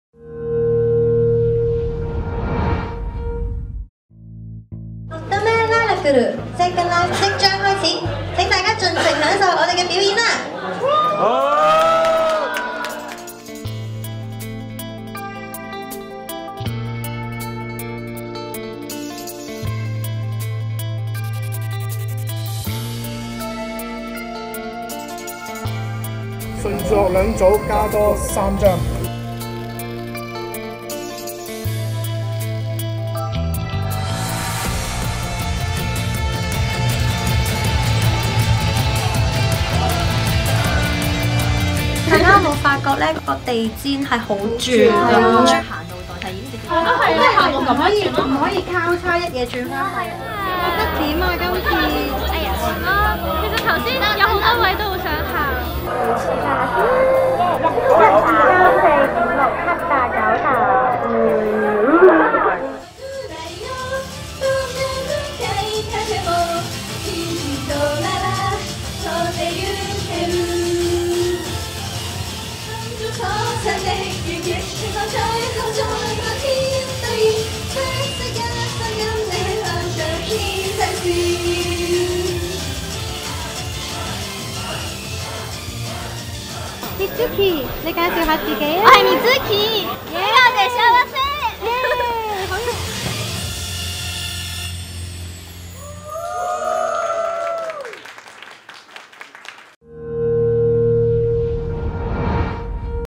港產日系偶像 - 乙女Lilacle Second Live
在寒冬的晚上，氣溫降至5度左右，在某座工業大廈中，有幾位少女穿著短袖制服和短裙，為台下面約200名觀眾，展現努力訓練多時的舞蹈，還有充滿希望的笑容。這個舞台和觀眾席的溫度絕對不止5度，他們的熱情和汗水令溫度上升。